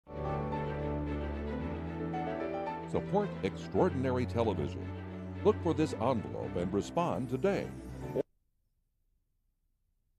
KAET Support Extraordinary Television (2006) “REST OF IT IS LOST”
This is one of the first KAET promos to have more than 5 seconds of the full instrumental heard in a bumper, the rest of the ad is lost because it was cut off at the end.